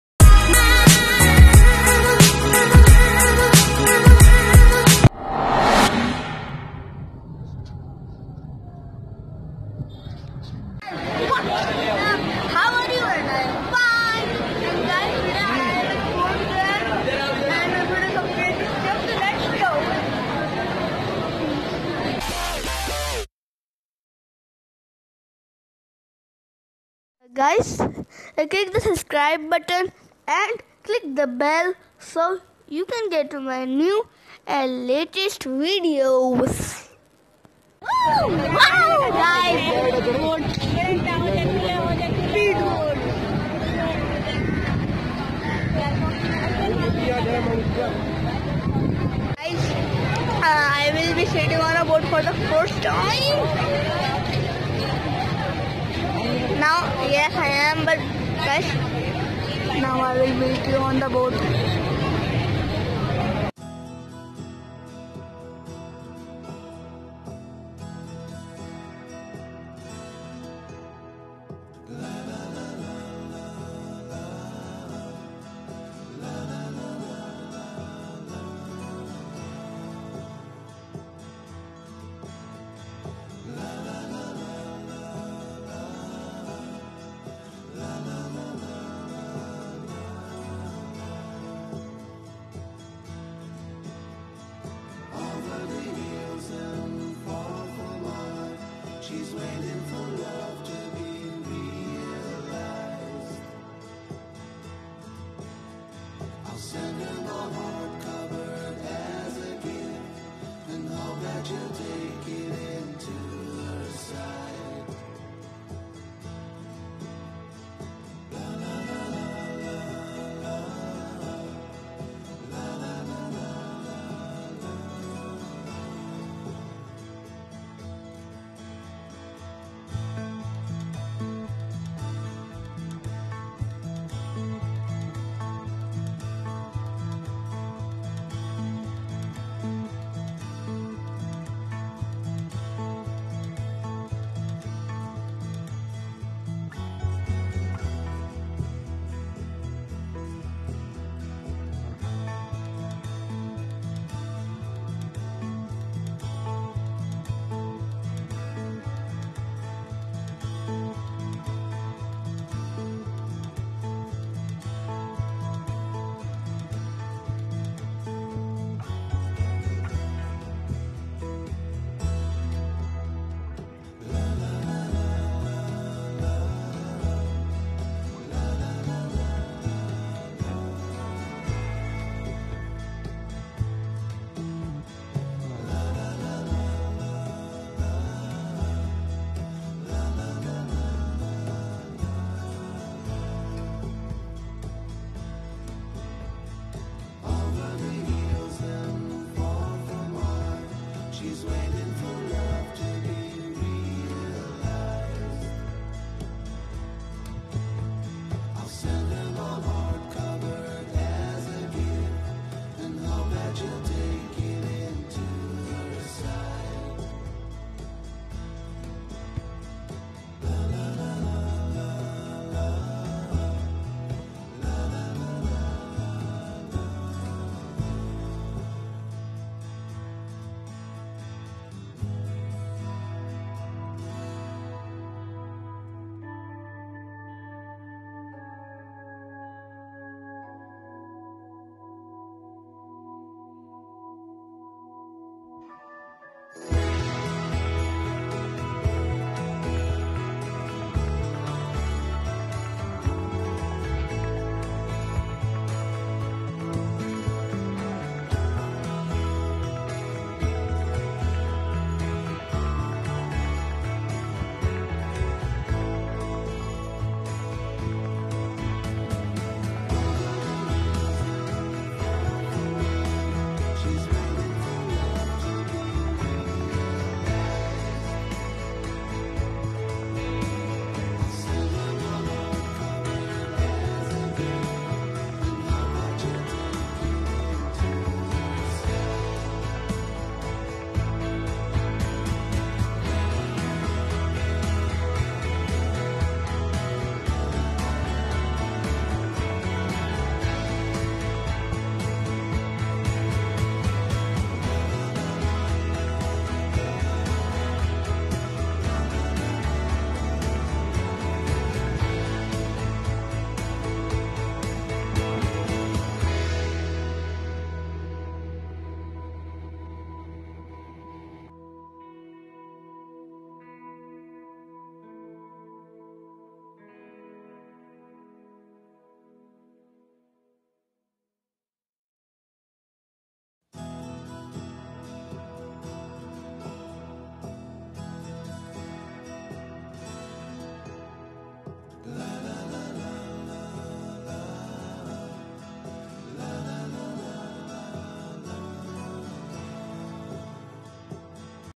BOAT RIDING
HI GUYS TODAY I WENT PORT GRAND AND DID BOAT RIDING IT WAS REALLY CALM AND RELAXING.